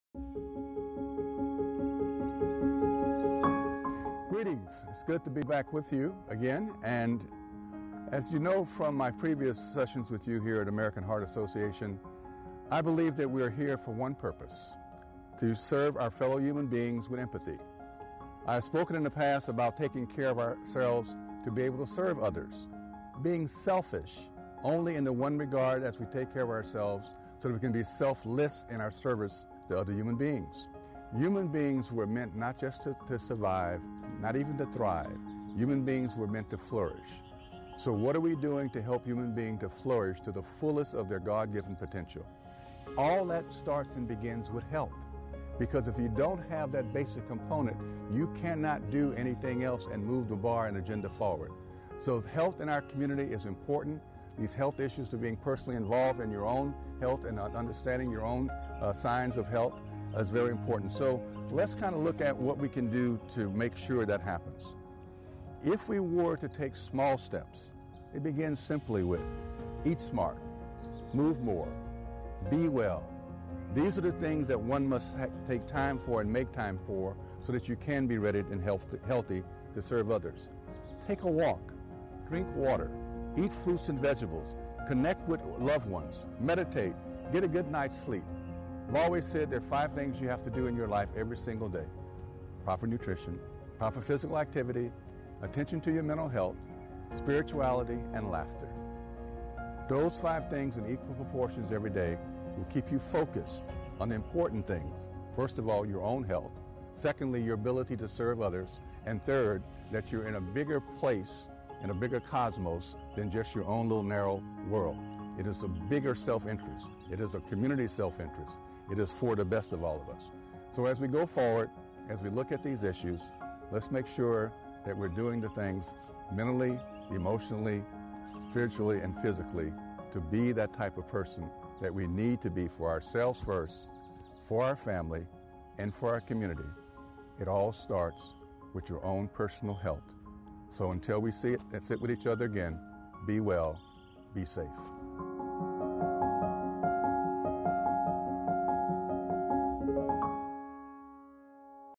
Larry Payne, Stroke Survivor and AHA Volunteer
"We are here for one purpose - to serve our fellow human beings with empathy. But we must also be selfish with regards to our own health so that we may be selfless in our service to others." - Larry Payne